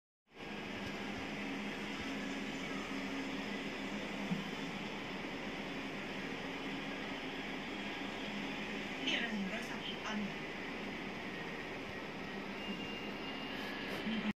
ถึงเวลาแม่นอนแล้ว...ยังไม่นอน
แม่ศรีนวลง่วงแล้วไม่ยอมไปนอน